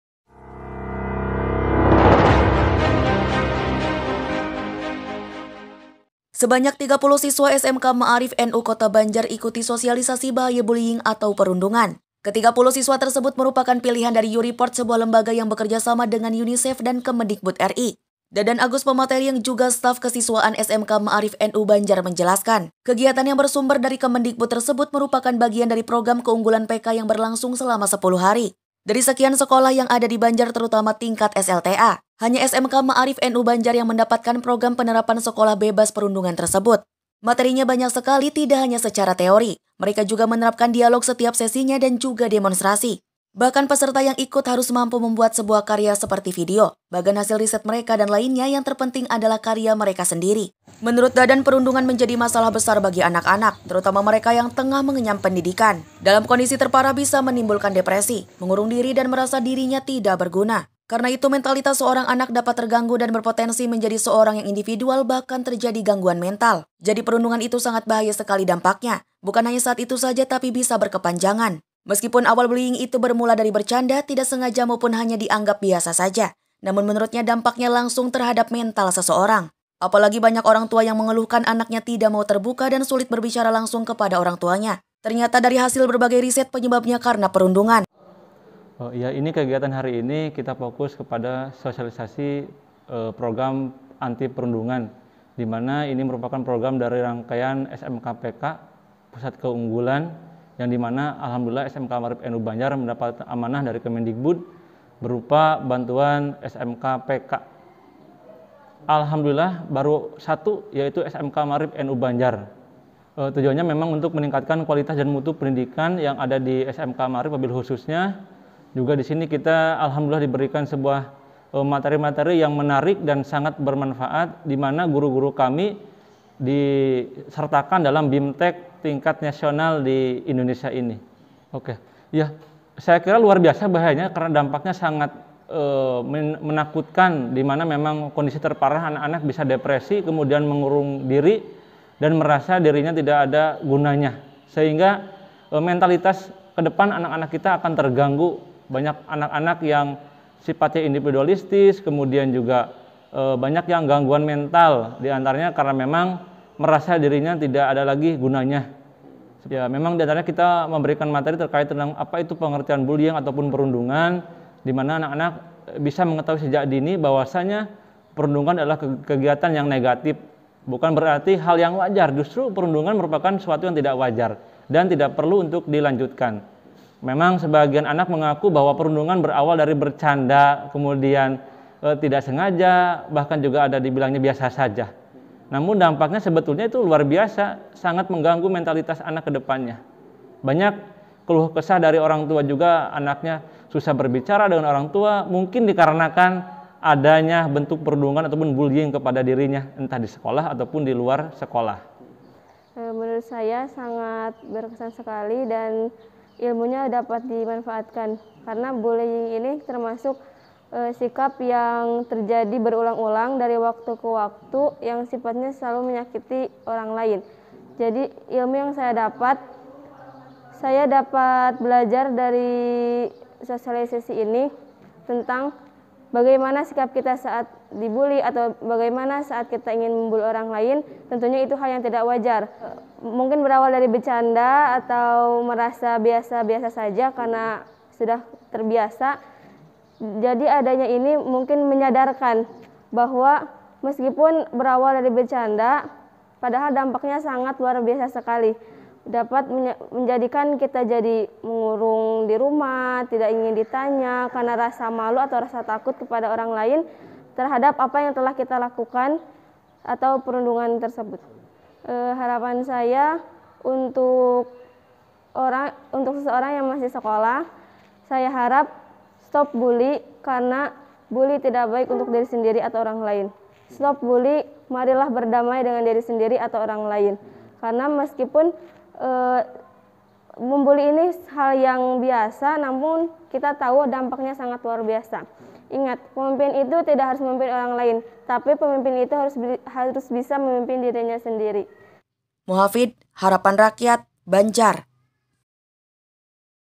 SMK Maarif NU Banjar Sosialisasikan Bahaya Bullying Kepada Siswa
Reporter: Muhafid
Editor: Muhafid
VO: Nita